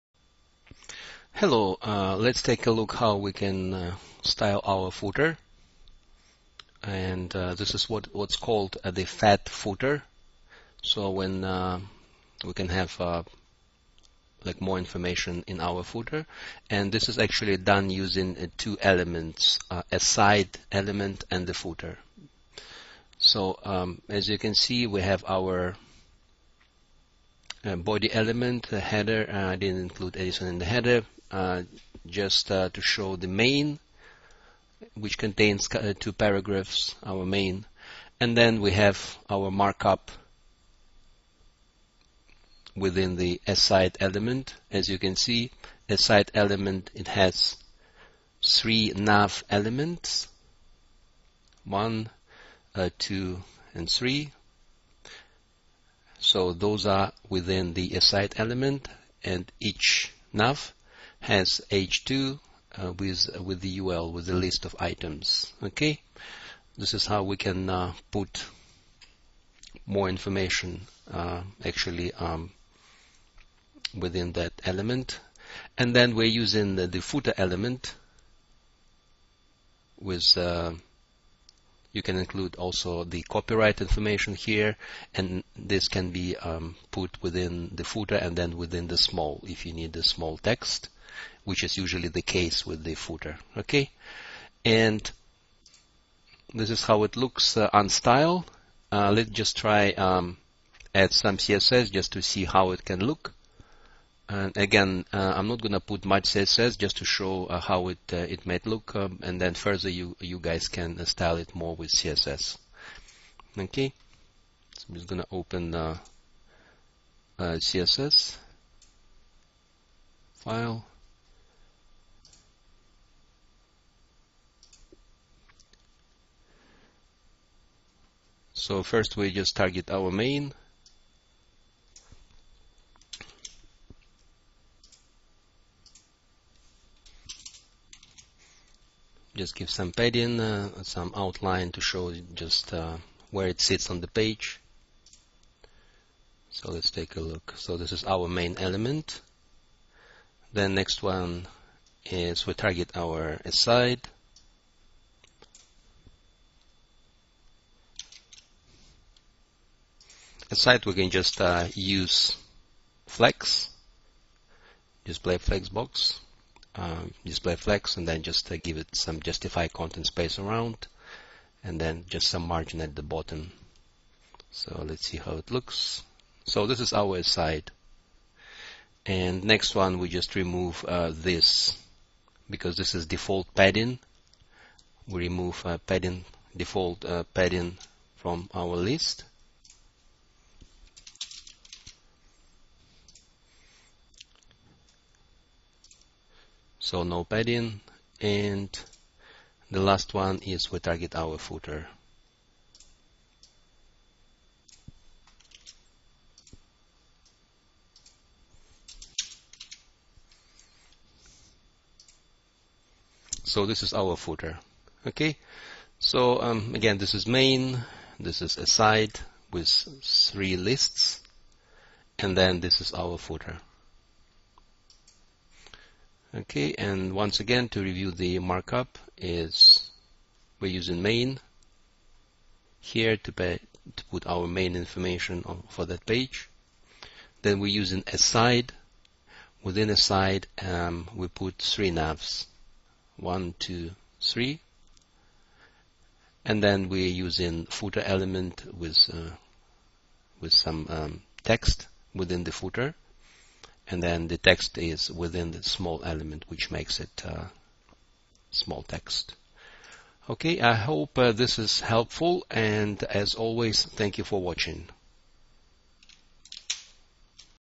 HTML - fat footer using aside and footer elements
We can create fat footer by actually using 2 HTML elements: aside and footer.